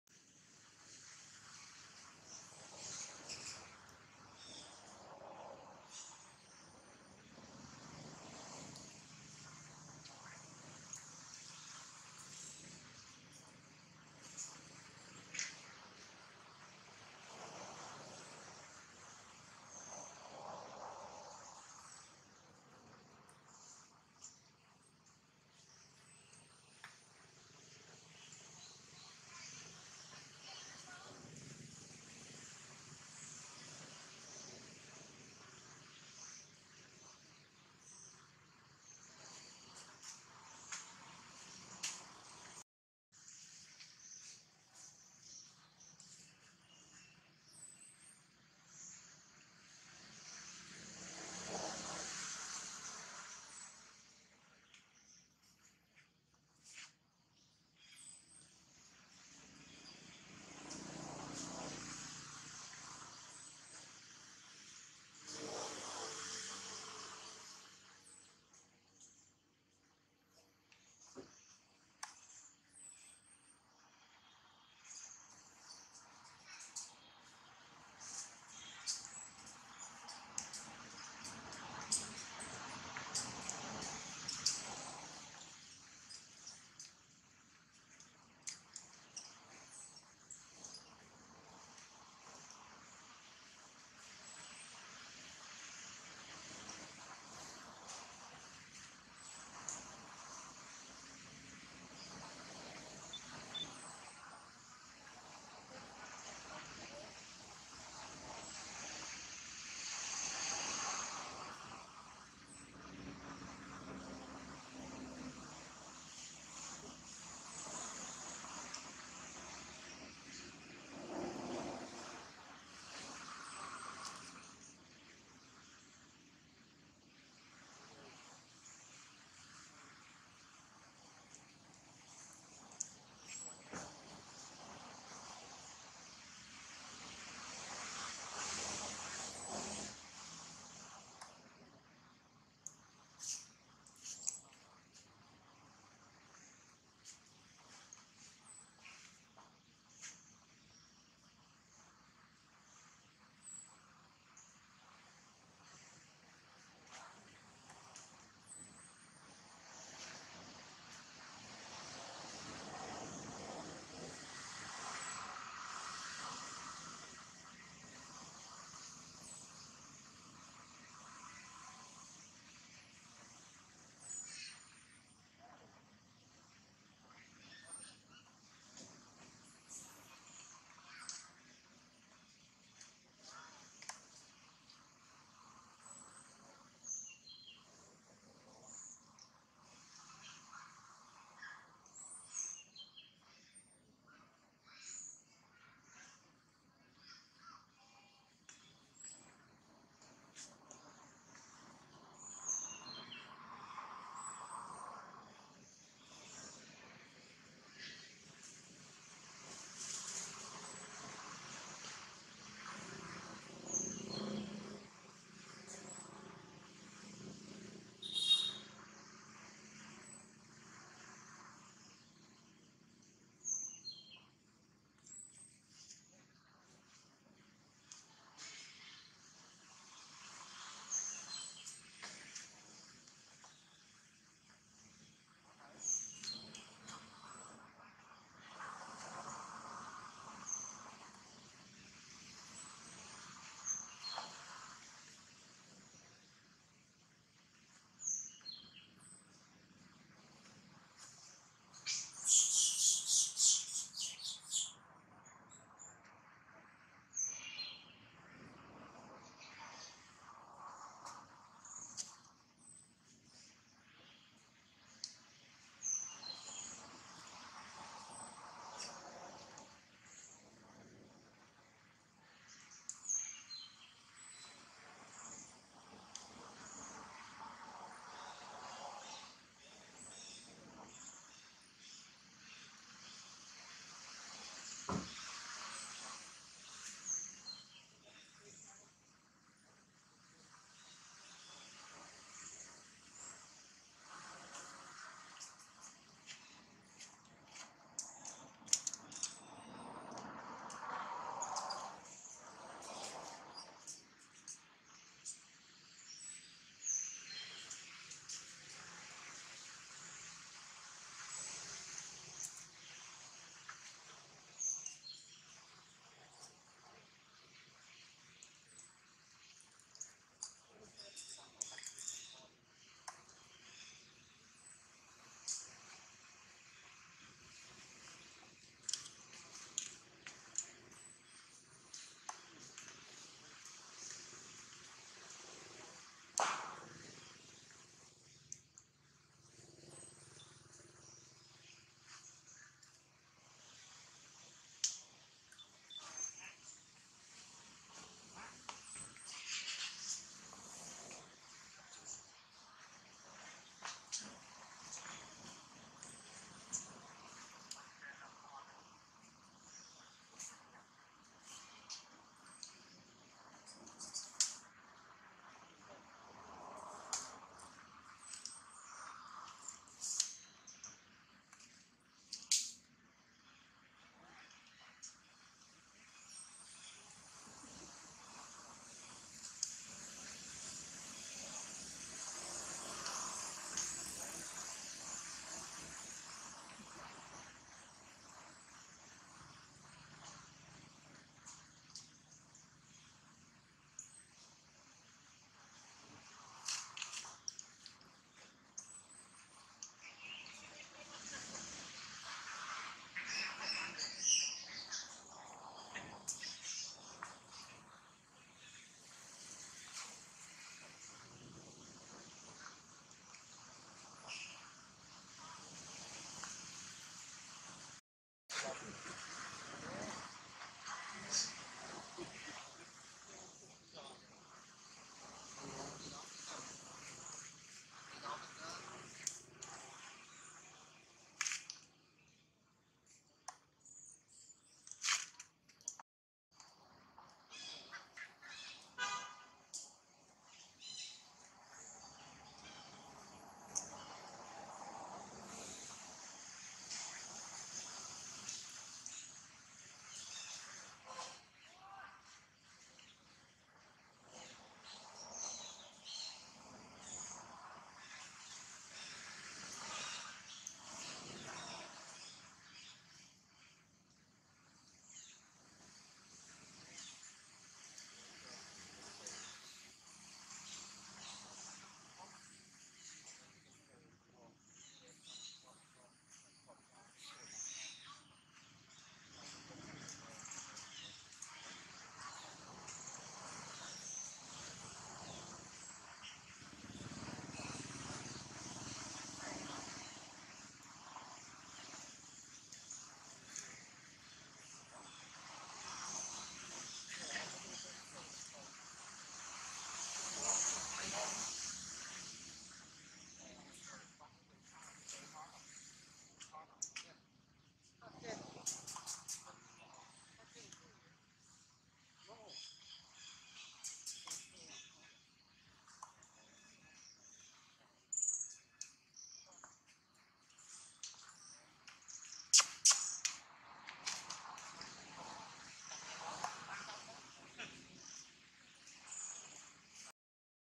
OMG ! Poor pregnant Jane still count down due date to baby born! can't wait to welcome her baby...
Everyday Monkey